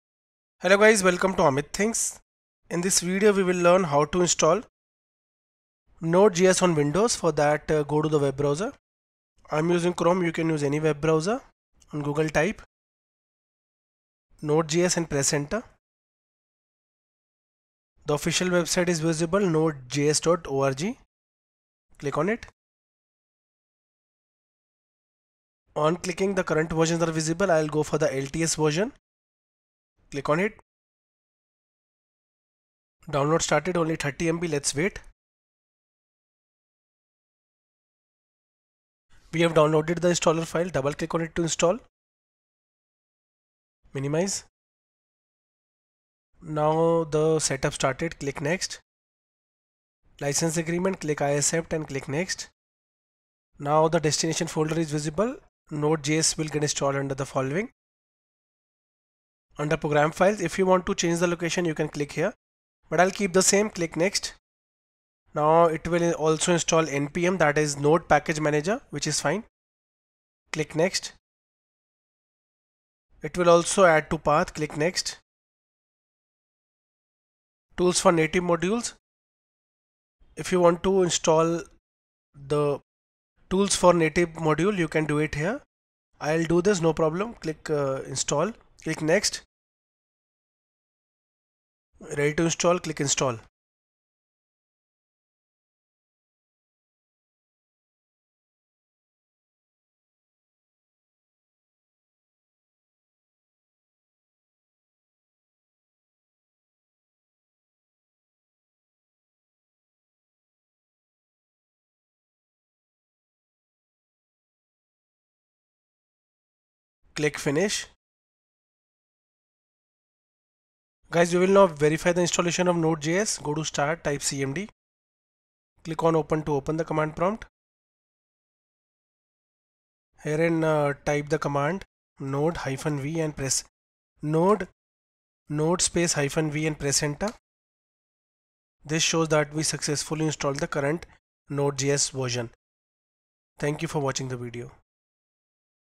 How to install Node.js on Windows 10/ 11 | Complete Installation| Amit Thinks
In this video, learn how to download and install Node.js on Windows 10/ 11.